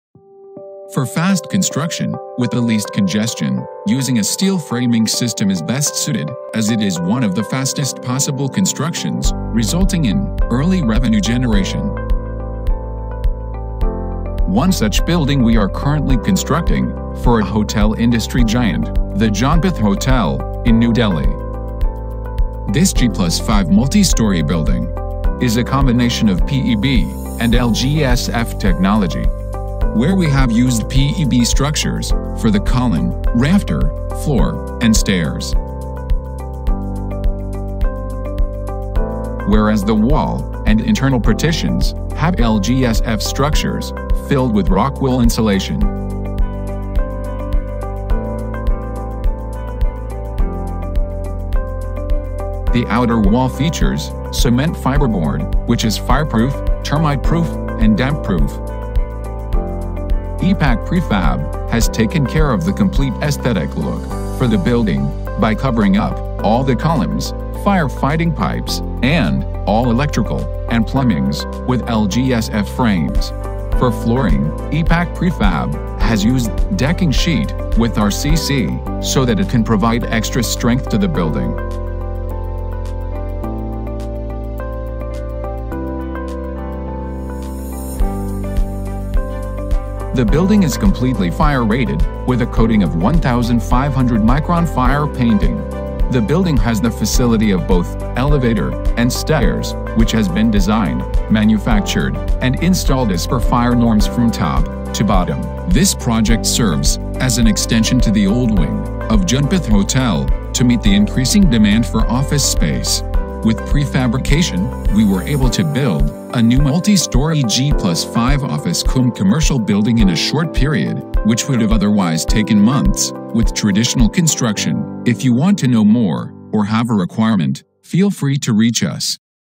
5 Floor Pre Engineered Steel Structure Building | PART-1
This  G+5 multi-story Pre Engineered Steel Structure Building, is a combination of PEB, and LGSF Technology. We have used  PEB Structures, for the column, rafter, floor, and stairs,  whereas the wall, and internal partitions, have LGSF structures, filled with Rockwool insulation,.
The outer wall features, cement fiberboard, which is fireproof, Termite Proof, and Damp Proof,. EPACK Prefab has taken care of the complete aesthetic look,
 for the building, by covering up, all the columns, Fire Fighting Pipes, and, all electrical, and plumbings, with LGSF frames.
For Flooring, EPACK Prefab has used,  Decking Sheet, with RCC,  so that it can provide extra strength to the building.
The Pre Engineered Building is completely fire-rated, with a coating of 1500 micron, fire painting. The Building has the facility of both, elevator, and stairs, which has been designed, manufactured, and Installed as per fire norms from top to bottom.